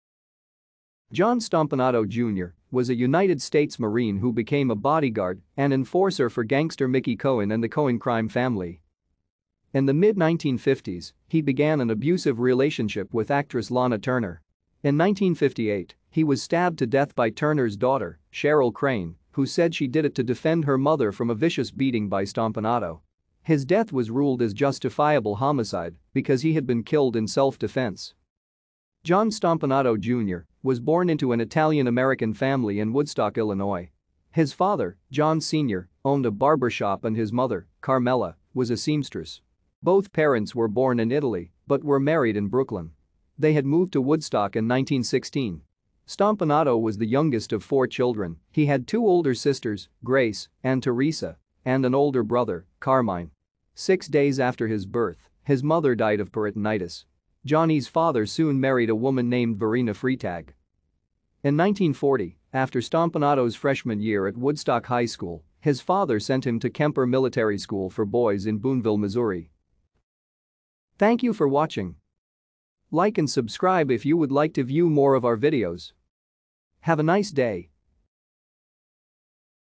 Johnny Stompanato
John Stompanato Jr., was a United States Marine who became a bodyguard and enforcer for gangster Mickey Cohen and the  Cohen crime family.